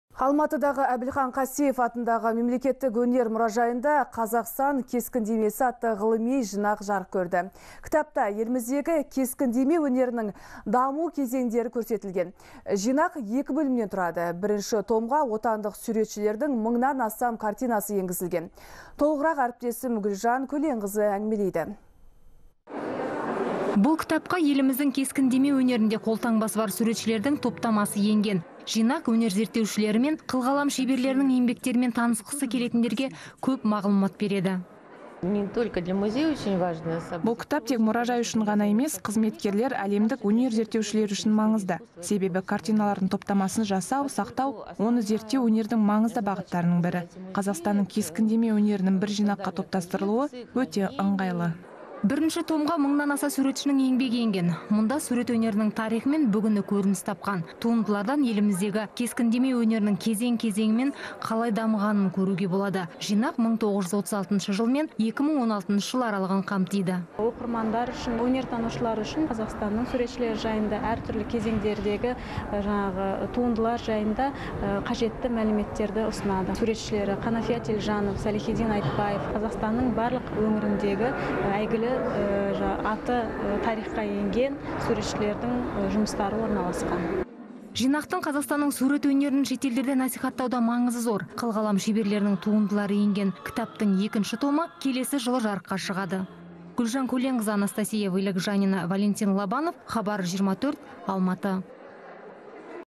Отандық суретшілердің 1000-нан астам картинасы «Қазақстан кескіндемесі» жинағына енгізілді
Алматыдағы Әбілхан Қастеев атындағы Мемлекеттік өнер мұржайында «Қазақстан кескіндемесі» атты ғылыми жинақ жарық көрді.